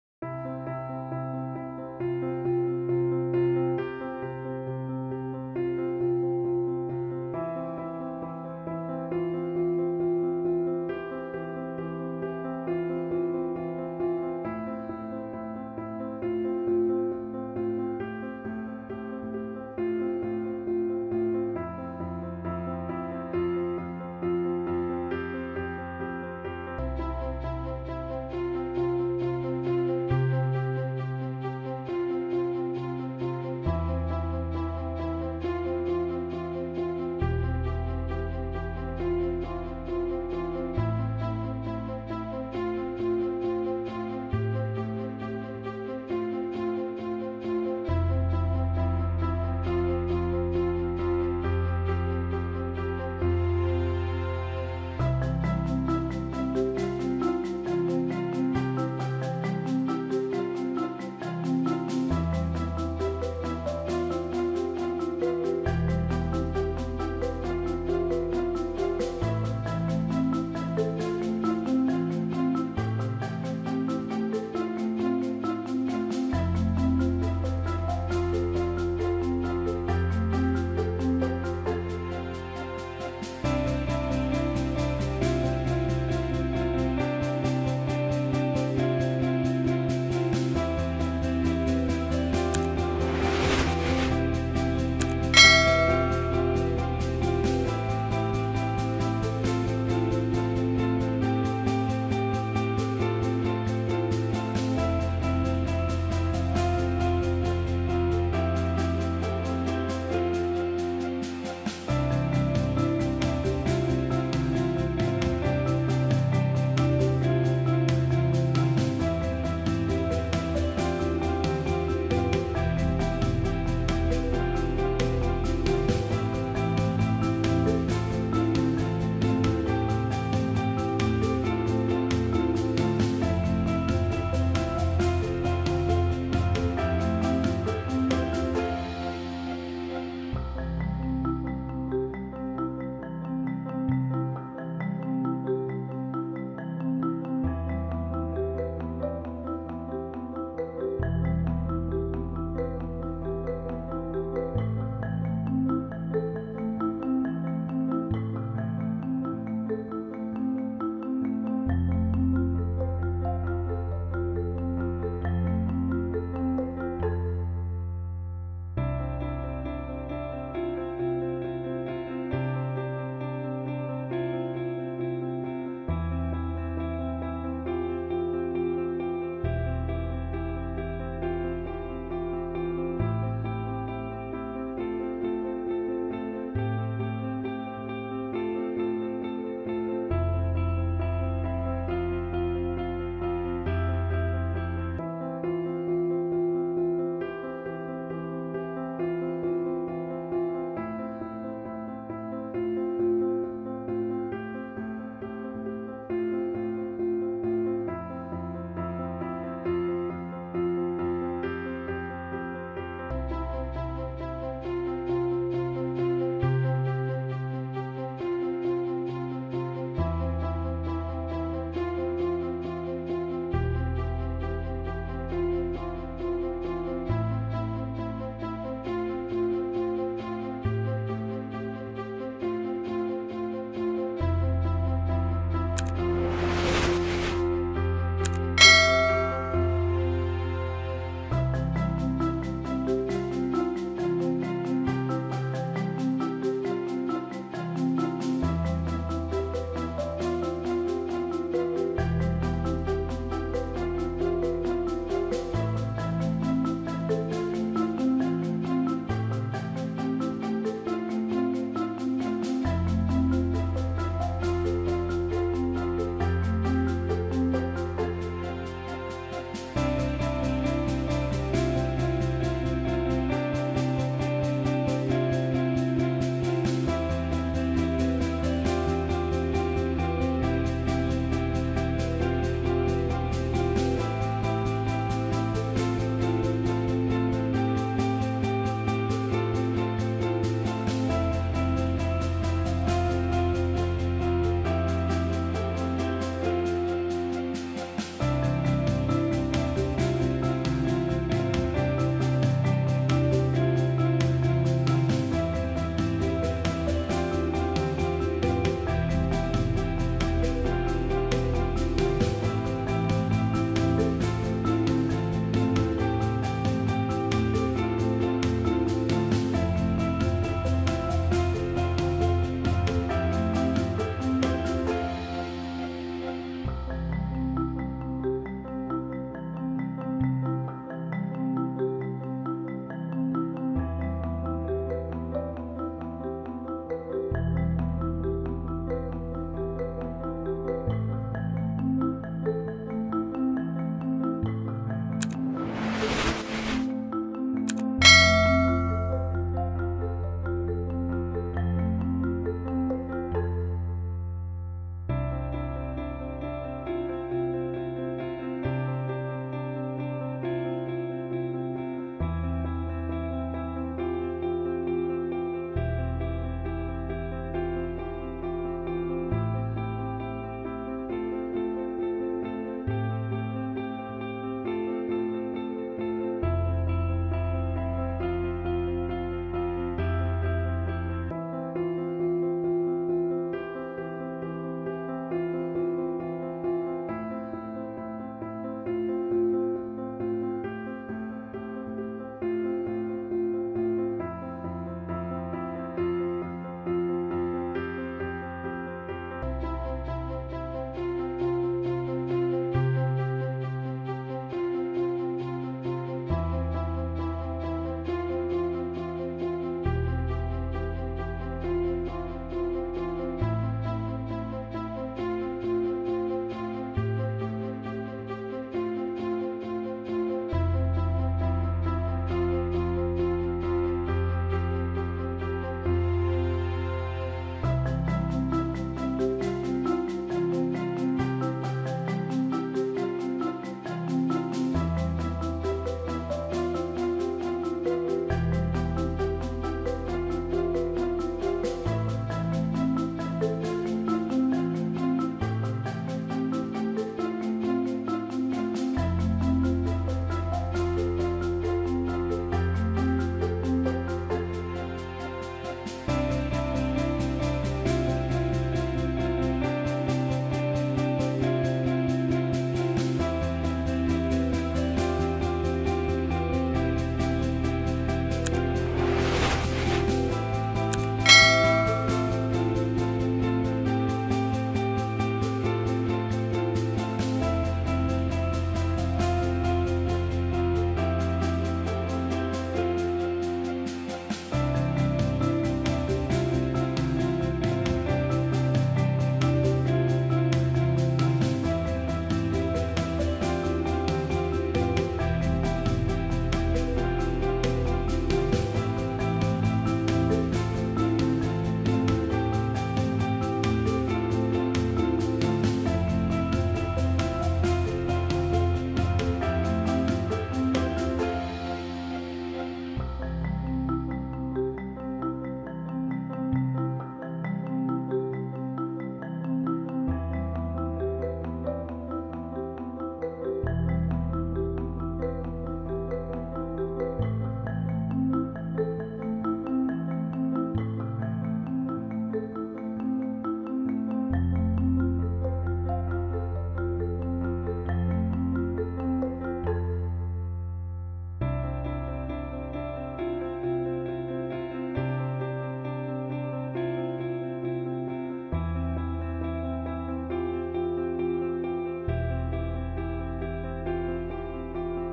Product Designing Tutorial - How To Make Chips , Snacks Packing Design illustrator with Photoshop
Product Designing Tutorial - How To Make Chips , Snacks Packing Design illustrator with Photoshop 1 for school students, college students, you tube viewers, you tube developers, job seekers